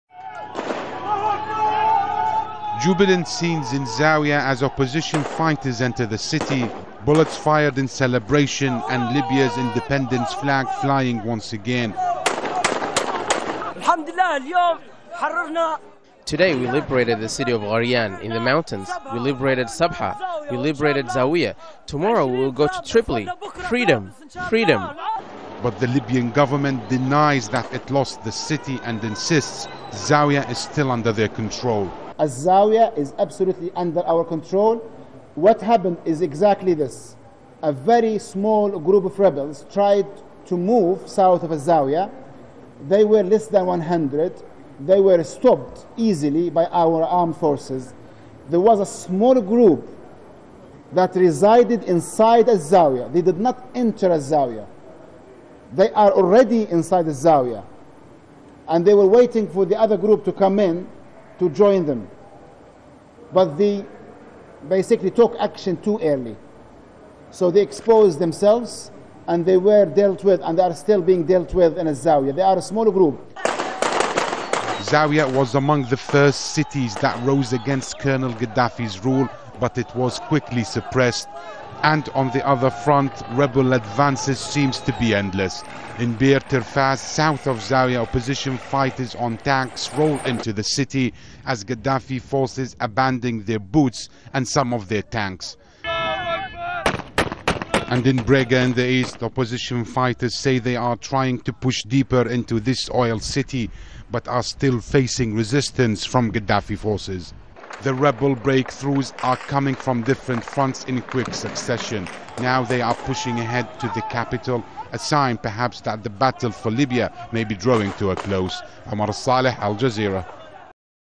AJE: Battle for Libya opposition advance on several fronts
Rebel fighters in Libya say they are within 50 kilometers of the country's capital, Tripoli.

Fighting continues between opposition forces and soldiers loyal to Muammar Gaddafi in the city of Az-Zawiyah, which is on the coastal road towards Tripoli.

The opposition say they are trying to stop supply routes to Gaddafi's forces in the capital.

Al Jazeera's Omar Alsaleh has the latest developments.